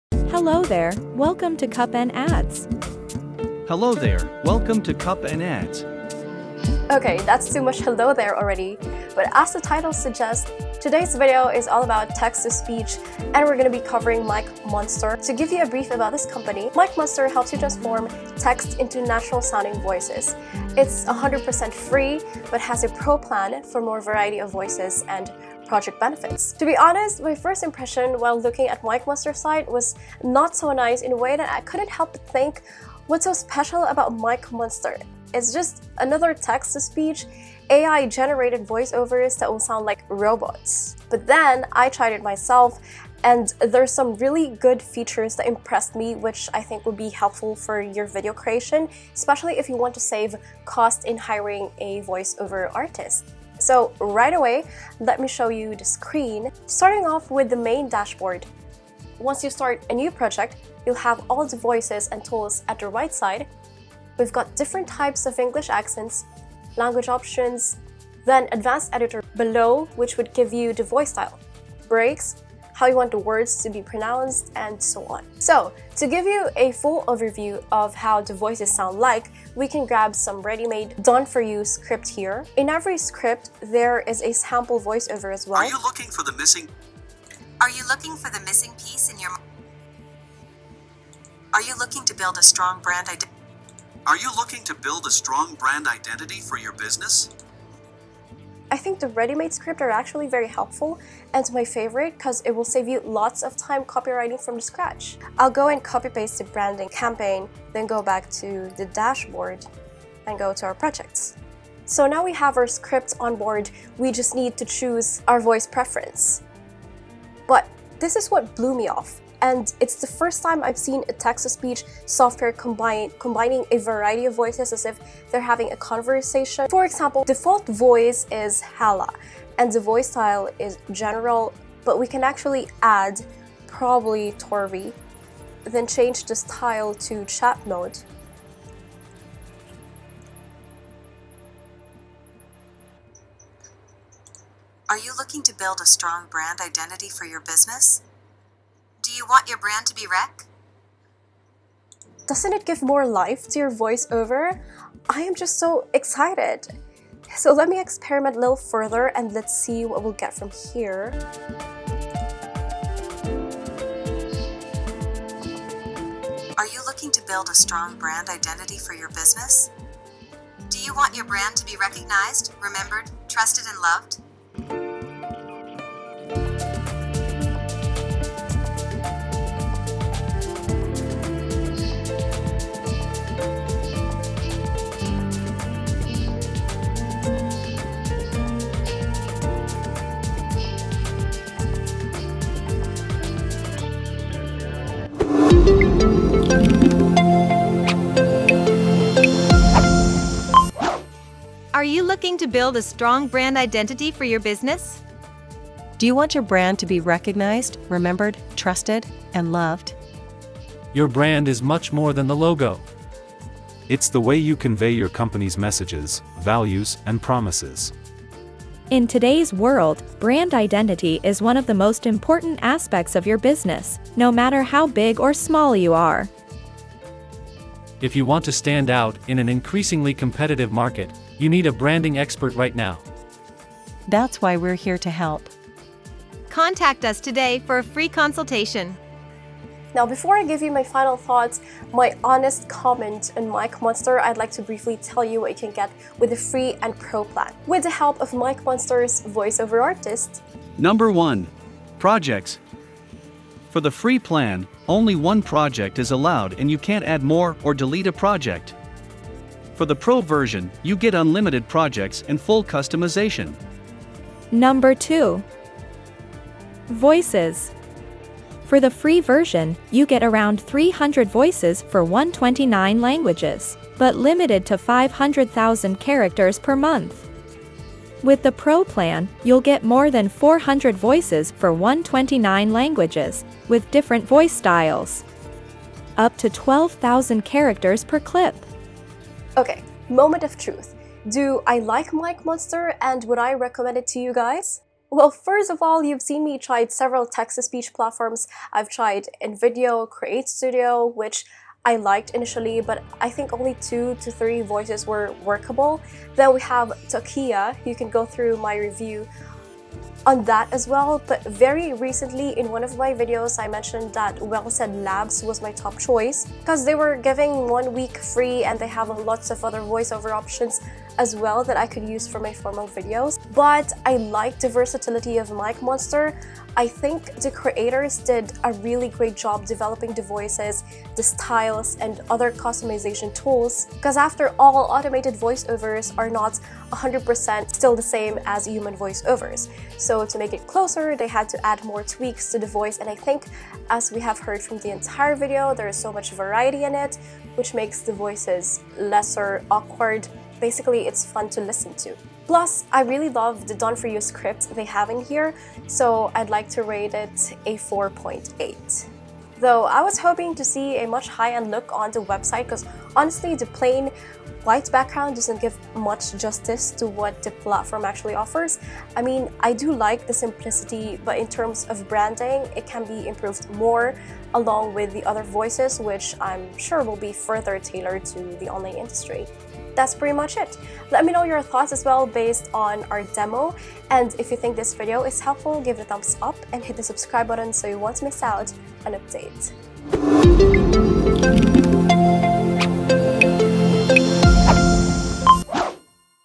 MicMonster Text to Speech: Honest Review
MicMonster Text to Speech: Honest Review and Demo

MicMonster helps you transform text into natural-sounding voices.

In this demo and full review, I went through some of the best features of Mic Monster and the AI generated voices.

But the main question is, is it really the best text to speech software as others claim it? And would I recommend it or not?